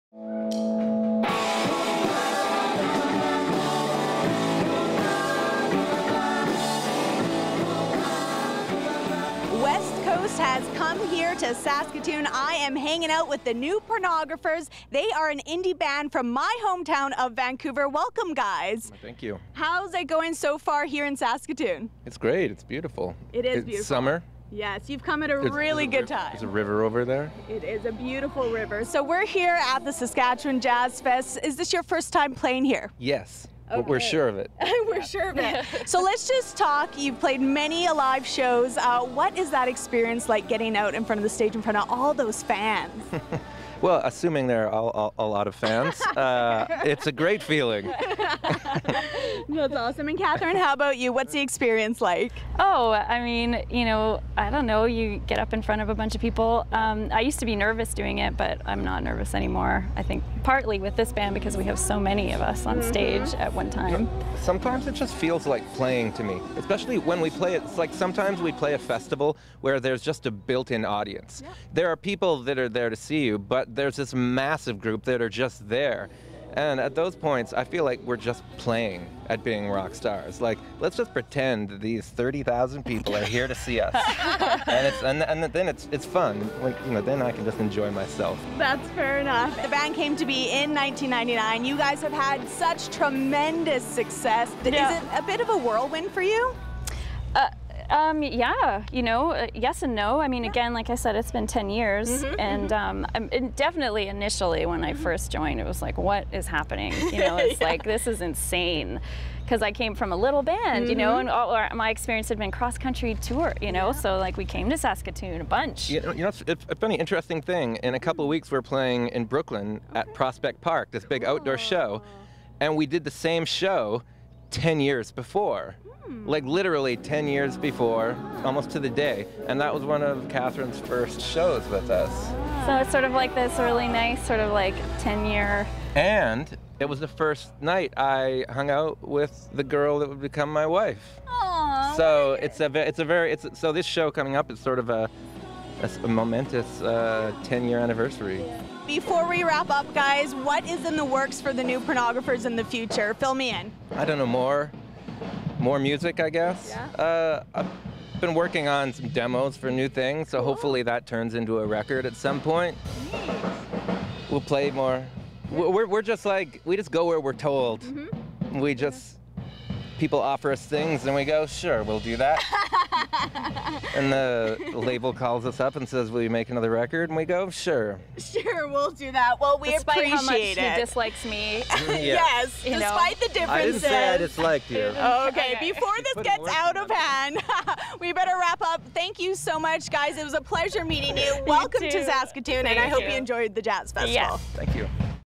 The New Pornographers
We chat with Vancouver band The New Pornographers.